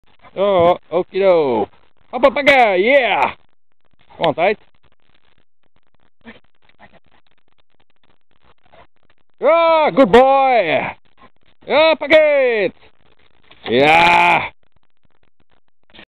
Pitbull jumping high. part 2
Titan is 14 months in this vid.
Rec.6 Mei '13.